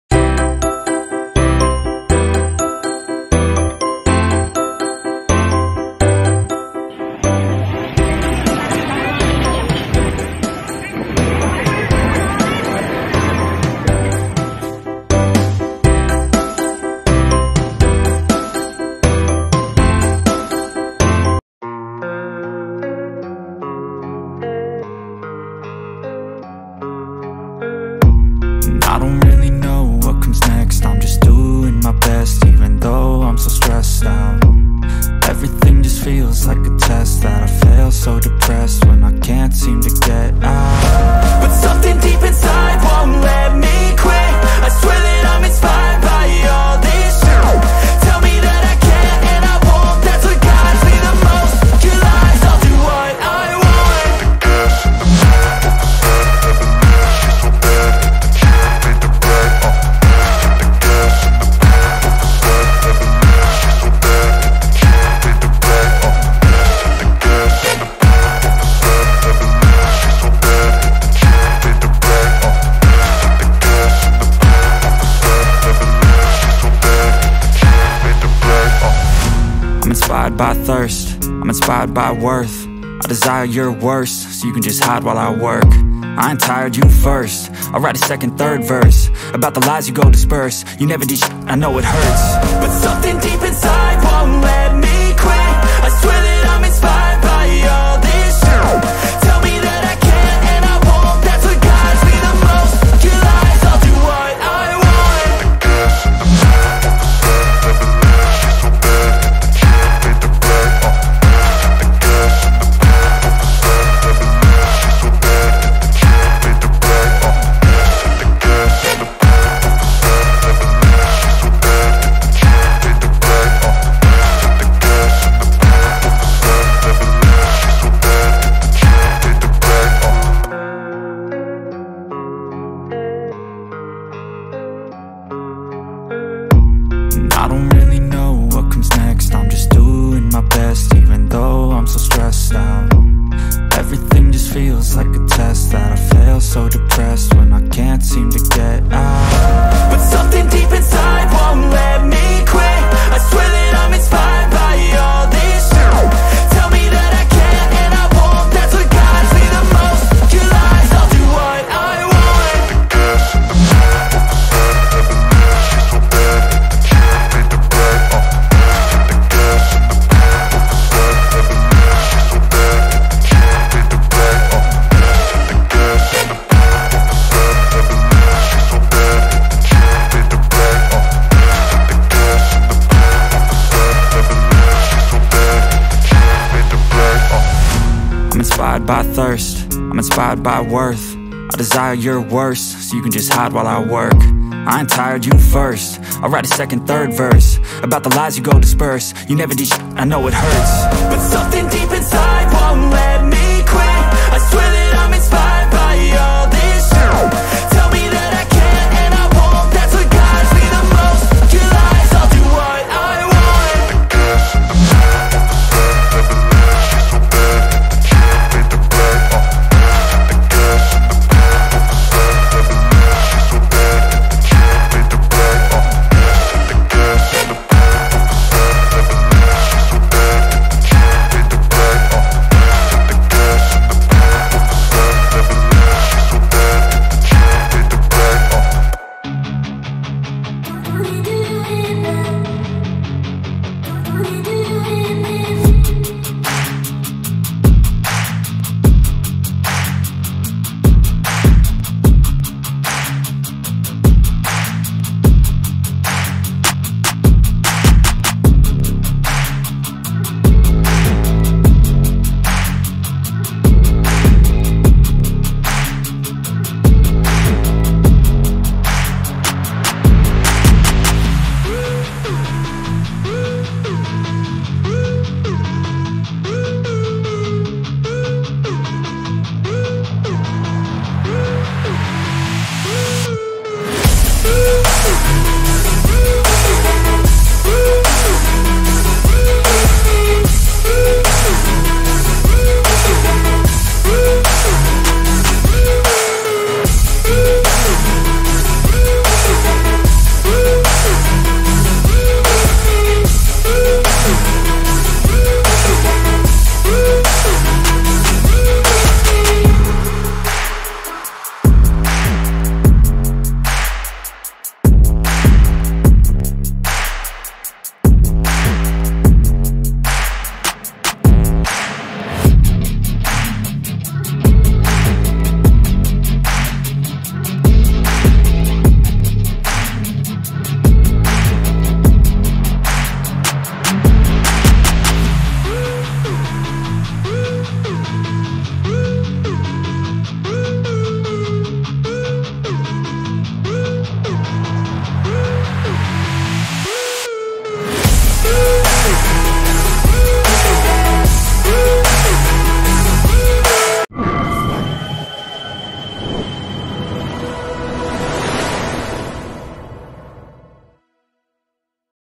SERVIS HP JADUL DIMONGKOK
Assalamualaikum 🙏

Hallo teman-teman
Kembali lagi di channel ku
 Hari ini lagi otw gaes .servis handphone yg lagi rusak.

Tepatnya dimongkok  gaes ,plaza hollywood no 20 F

Ayo temen-teman ikuti keseruan ku ya .

Ikuti keseruan ku ya teman-teman

Alat edit  ✓

Handphone :  redmi note 10

Aplikasi edit video : inshot

Backsound : YouTube audio libary

Dan jangan lupa untuk

Terimakasih  atas supportnya🙏

Waalaikumsalam 🙏